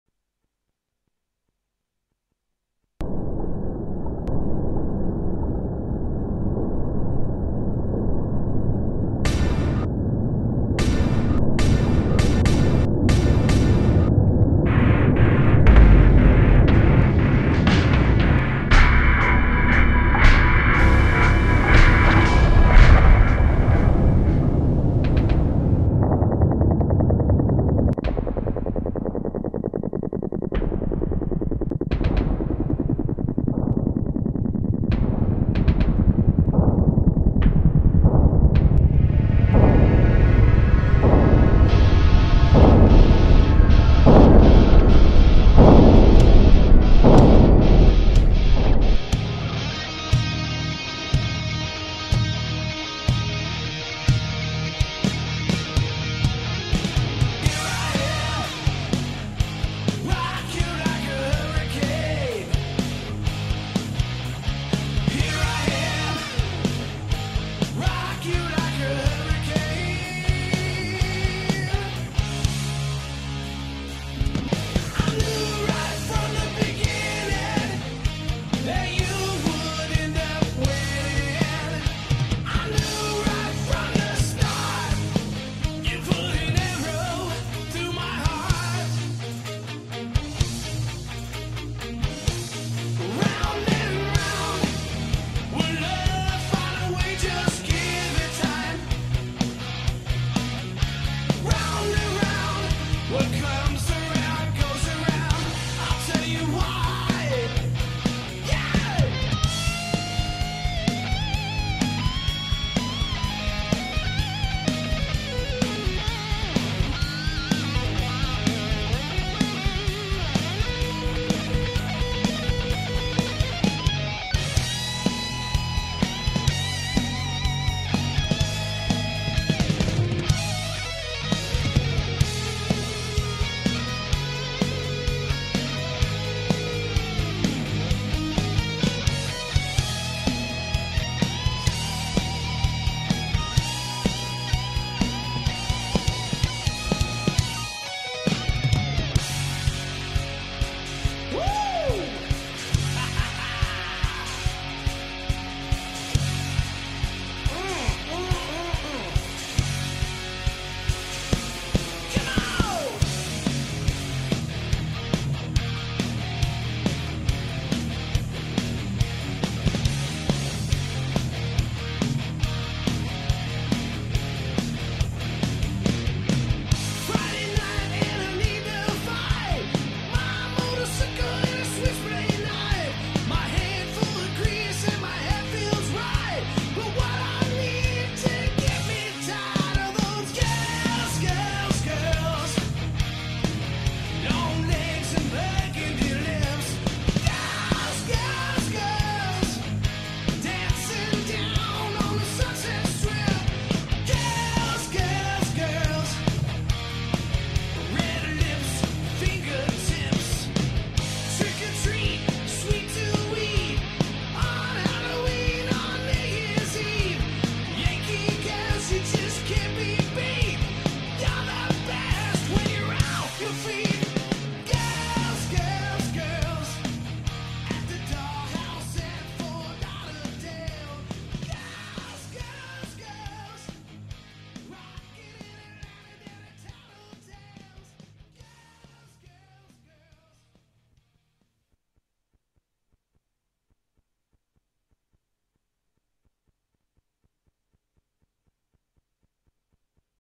Love Pump Promo
The almighty LOVE PUMP in action!
Kansas City's Tribute to 80's Hard Rock and Metal!!!